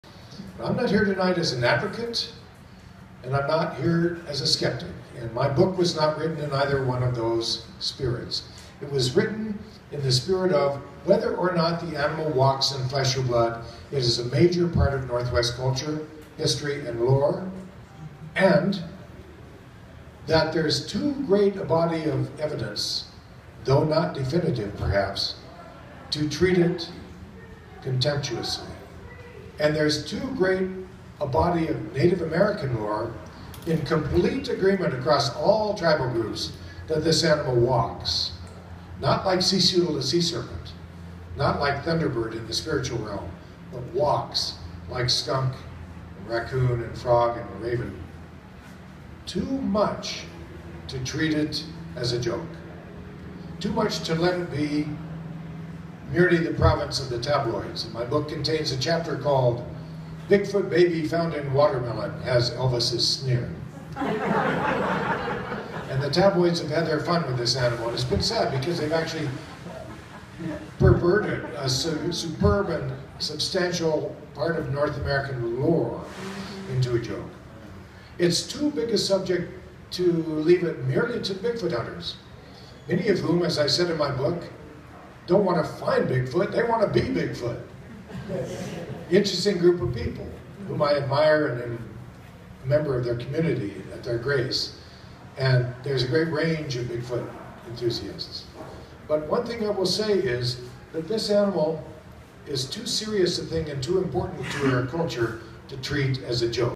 Dr. Robert Michael Pyle Presents "Of Butterflies and Sasquatch"
May 20th 2014. Dr. Robert Michael Pyle presents "Of Butterflies and Sasquatch". he takes us through the wildlands around Mount St. Helens, which are famous for the abundance of Bigfoot reports they have generated. Fewer people may be aware that the Gifford Pinchot National Forest is also good butterfly country. Dr. Pyle has researched and written extensively about both of these phenomena.